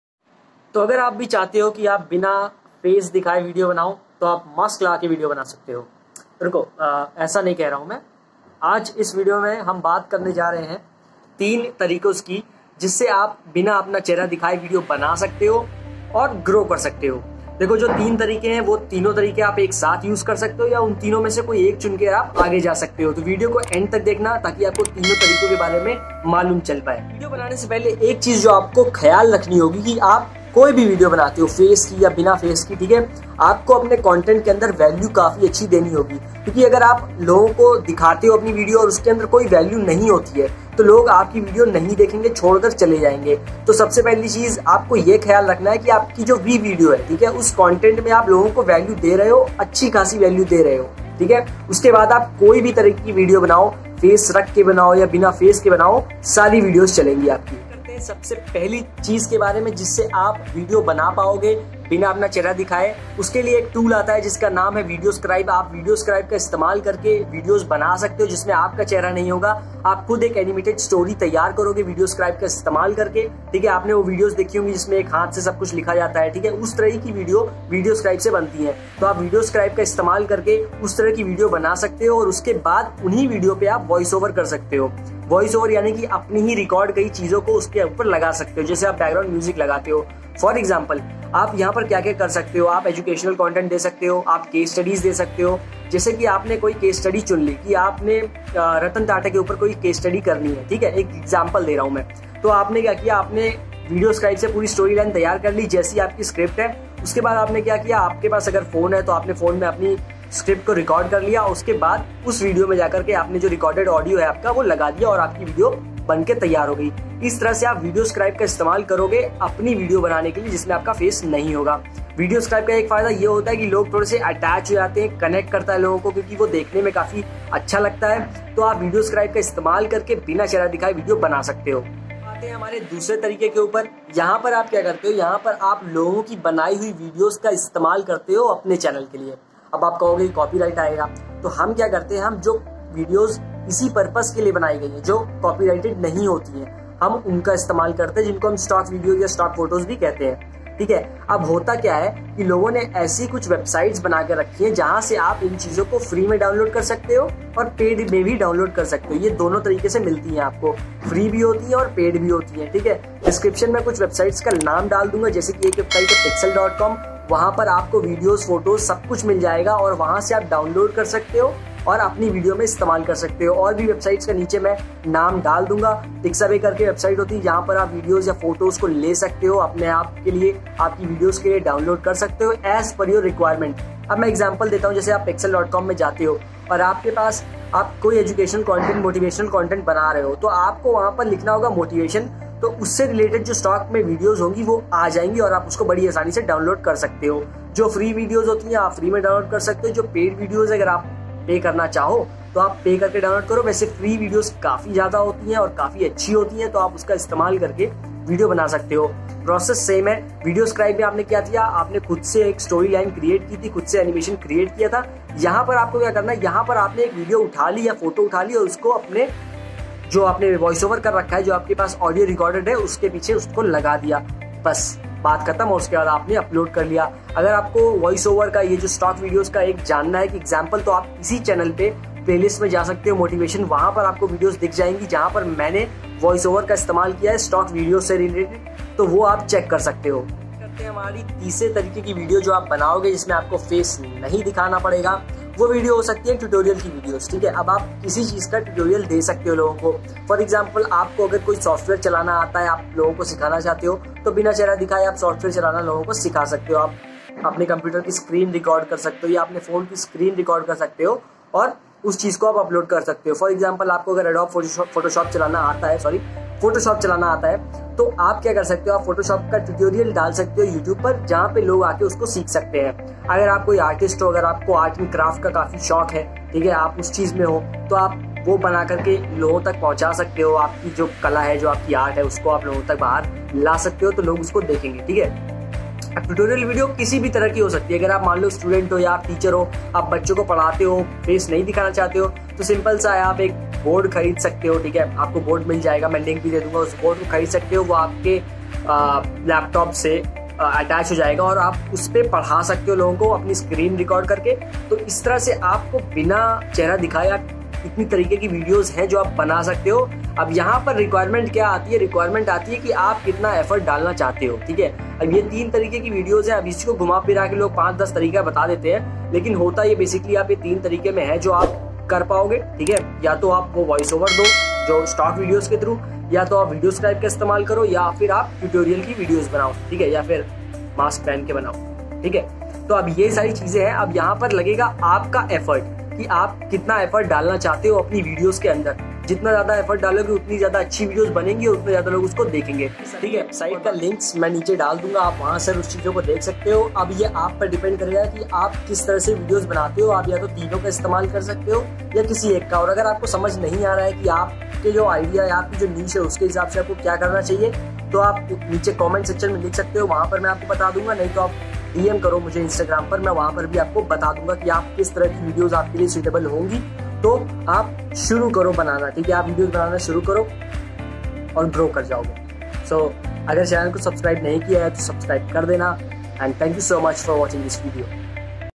HOW TO MAKE YOUTUBE VIDEOS WITHOUT SHOWING FACE | BINA CHEHRA DIKHAE VIDEO KAISE BANAE IN HINDI
In this video i have talked about on how to make videos without showing your face. If you have any problem in showing your face then this video will definitely help you.

STAY SAFE

//RS//
youtube videos without showing face
how to grow a youtube channel fast
how to grow my youtube channel